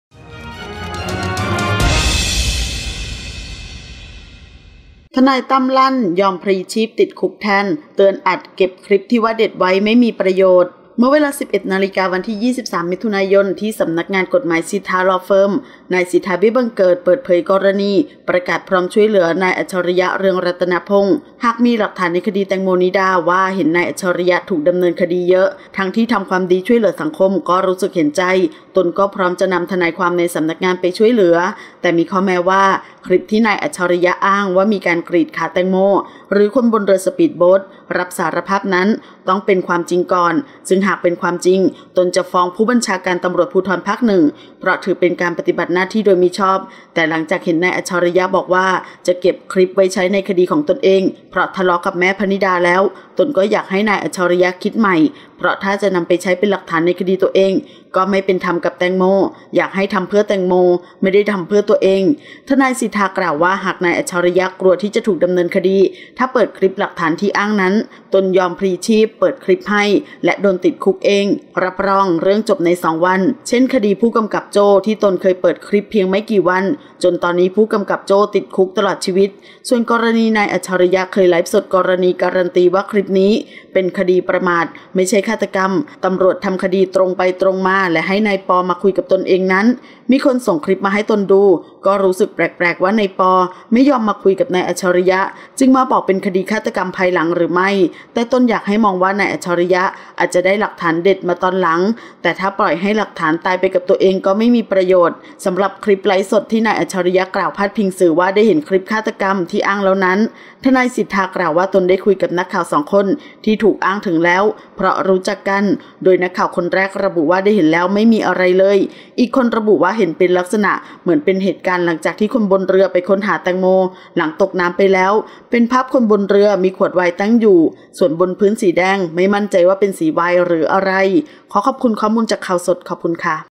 ทนายตั้ม ลั่น ยอมพลีชีพติดคุกแทน เตือนอัจเก็บคลิปที่ว่าเด็ดไว้ไม่มีประโยชน์
เมื่อเวลา 11.00 น.วันที่ 23 มิ.ย.ที่สำนักงานกฎหมาย ษิทราลอว์เฟิร์ม ทนายษิทรา เบี้ยบังเกิด หรือทนายตั้ม เปิดเผยกรณีประกาศพร้อมช่วยเหลือนายอัจฉริยะ เรืองรัตนพงศ์ ประธานชมรมช่วยเหลือเหยื่ออาชญากรรม หากมีหลักฐานในคดีแตงโม นิดา ว่า เห็นนายอัจฉริยะ ถูกดำเนินคดีเยอะ ทั้งที่ทำความดีช่วยเหลือสังคมก็รู้สึกเห็นใจ ตนก็พร้อมจะนำทนายความในสำนักงานไปช่วยเหลือ

แต่มีข้อแม้ว่า คลิปที่นายอัจฉริยะ อ้างว่ามีการกรีดขาแตงโม หรือคนบนเรือสปีดโบ๊ทรับสารภาพนั้นต้องเป็นความจริงก่อน ซึ่งหากเป็นความจริง ตนจะฟ้องผู้บัญชาการตำรวจภูธรภาค 1 เพราะถือเป็นการปฏิบัติหน้าที่โดยมิชอบ แต่ตอนหลังเห็นนายอัจฉริยะ บอกว่าจะเก็บคลิปไว้ใช้ในคดีของตัวเอง เพราะทะเลาะกับแม่ภนิดาแล้ว ตนก็อยากให้นายอัจฉริยะ คิดใหม่ เพราะถ้าจะนำไปใช้เป็นหลักฐานในคดีตัวเอง ก็ไม่เป็นธรรมกับแตงโม อยากให้ทำเพื่อแตงโม ไม่ได้ทำเพื่อตัวเอง